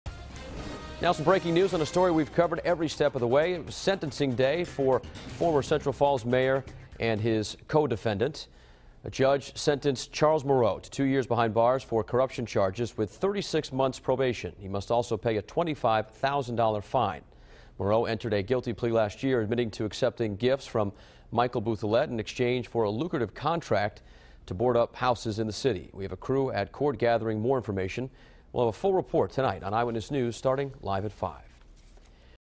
Former Central Falls Mayer sentenced for corruption charges
A judge sentenced Charles Moreau to two years in prison for corruption charges, with 36 month probation, as well as a 25 thousand dollar fine. Moreau admitted to accepting gifts from Michael Bouthilette in exchange for a lucrative contract to board up houses in the city.